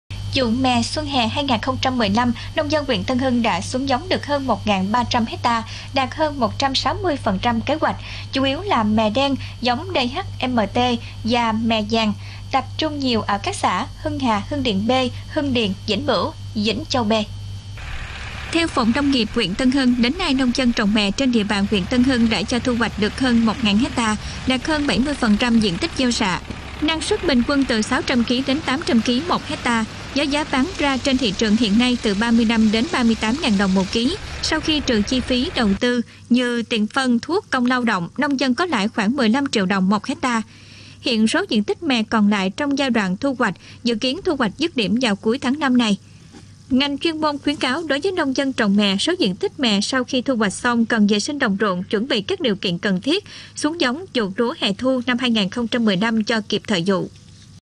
Giá mè cao, nông dân Tân Hưng lãi lớn | LATV
LATV | Nông dân Tân Hưng đã thu hoạch mè vụ xuân hè với năng suất lớn hơn mọi năm.

* Điện thoại: ( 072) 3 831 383